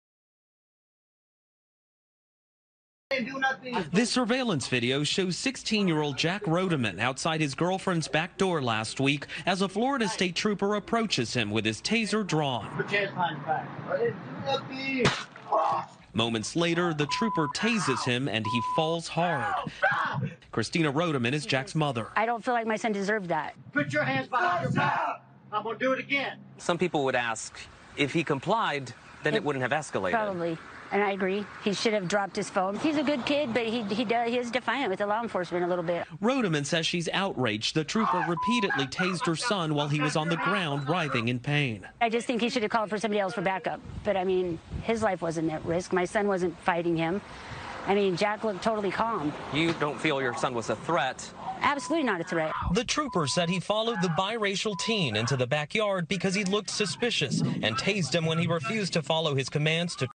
baller A 16-year-old boy was tasered by a patrol officer who follow
Biracial Teenager Tasered By Trooper For Looking “Suspicious”

Blogged by: @Cakedupdrippedout

A 16-year-old boy was tasered by a patrol officer who followed him to his girlfriend’s home because he “looked suspicious.”

On June 16th, JackRodeman, who is biracial, was confronted by GeorgeSmyrnios, a Florida Highway Patrol Trooper, in the backyard of his girlfriend’s residence in Indian Laurel Way, FortMyers. Surveillance video showed the shocking moment when the teenager was tasered, which caused him to fall back and hit his head on a brick fountain.

The trooper is seen on film pointing his taser at the teen and ordering him to place his hands behind his head moments before discharging the weapon.

“I didn’t do nothing,” Rodeman replies as he continues looking down at his cell phone. Once tasered, Rodeman screams in pain and continues to shout that he has not done anything. As the teen lay on the ground while the officer cuffed him, his girlfriend came outside the door and informed Smyrnios that her boyfriend was allowed to be at the home. However, he was still apprehended...to read the rest log on to BallerAlert.com (clickable link on profile)